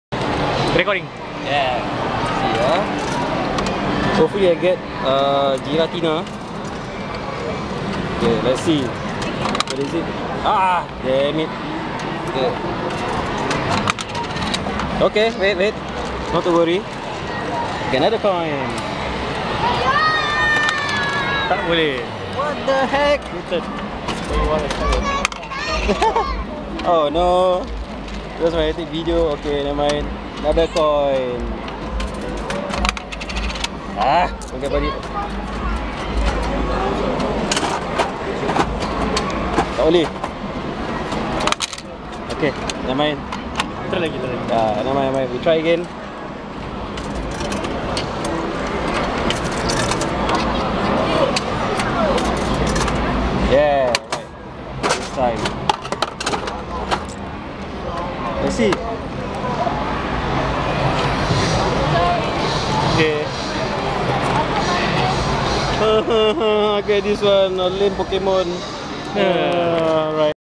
Gashapon Machine Playing At STGCC
My friend recording me playing the Gashapon machine at the annual Singapore Toys, Games and Comics Convention. Initially the machine refused to take in my coin. Took some time before it could accept it.

Up till now I wondered why of all places, I went to the one dispensing pokemon keychains...